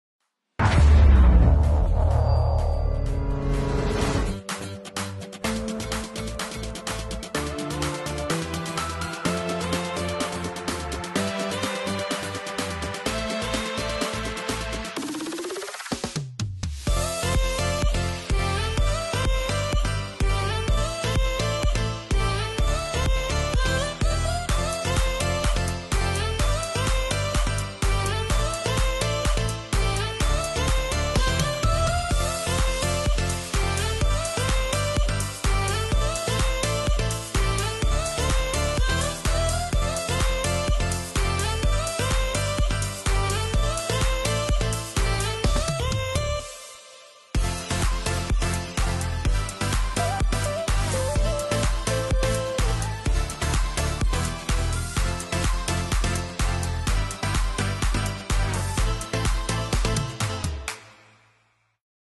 Speed Curtain VO 5467143
SR speedcurtain SchmitzCargobull disponible sur notre parc ctslyon St Quentin-Fallavier.

A essayer vite, les rideaux s'ouvrent et se referment en 36 secondes! :-)